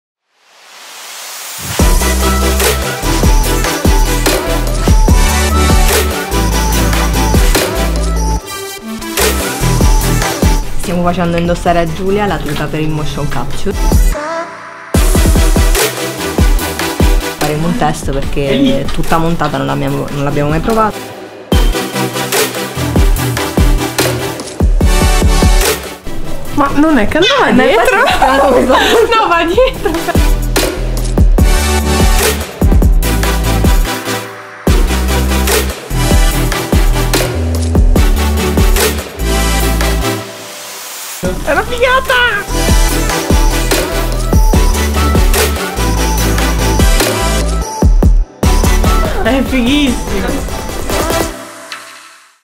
Proviamo la nuova tuta MOCAP
Primi test con i RED della tuta per il mocap.